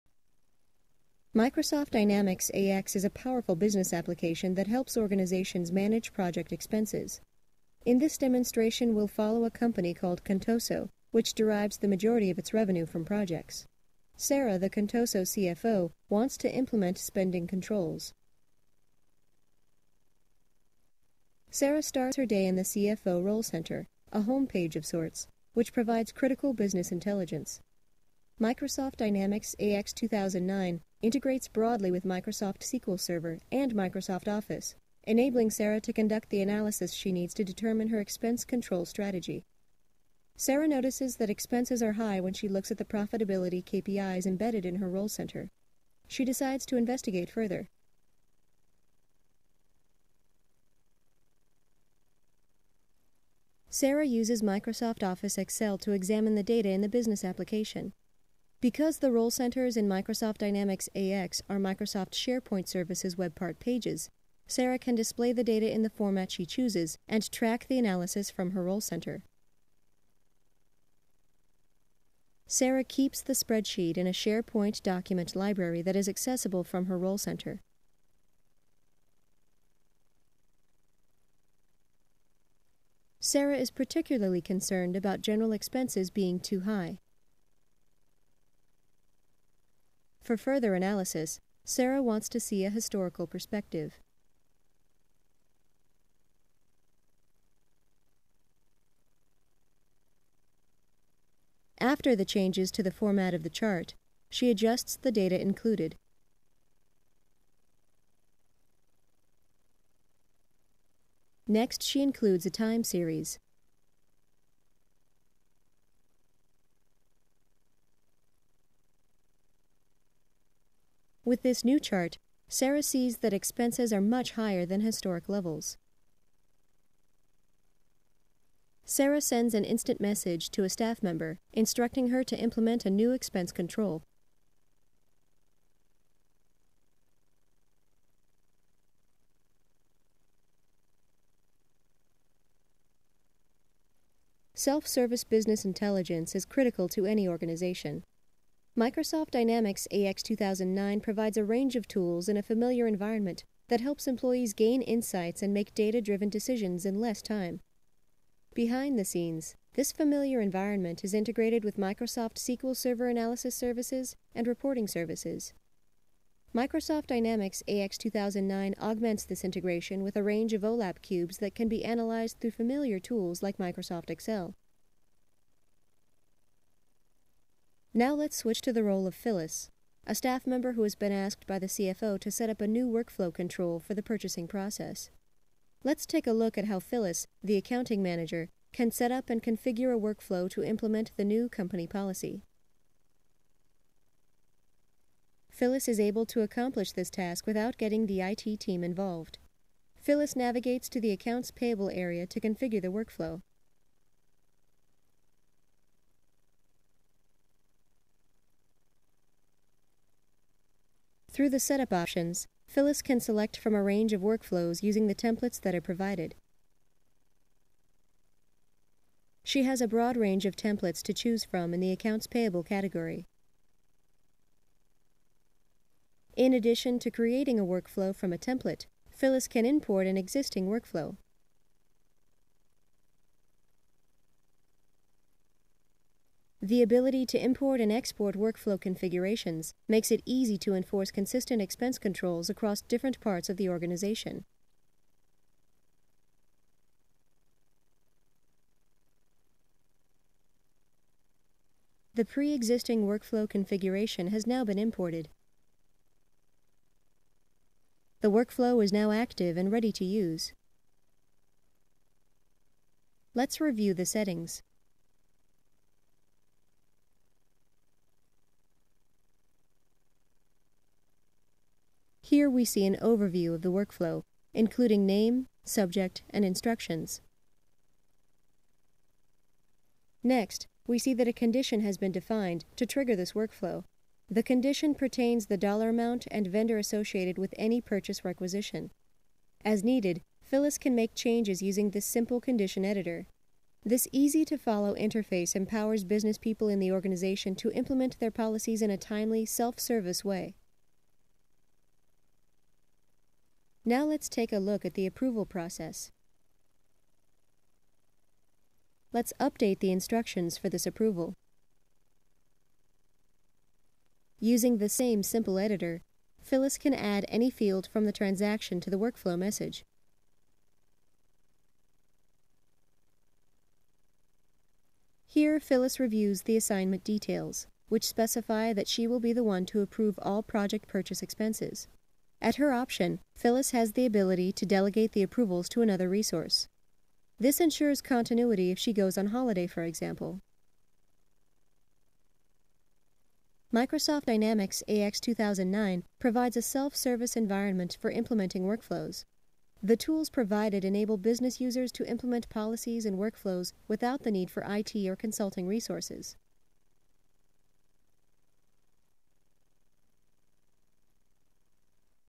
Microsoft Dynamics AX: Proj Expenses (Part 1 of 2)
"In this demonstration, you'll see how Microsoft Dynamics AX provides access to critical business information in a self-service environment via the Role Center feature.  You'll also see how users can leverage automated workflows to implement policies and manage internal controls.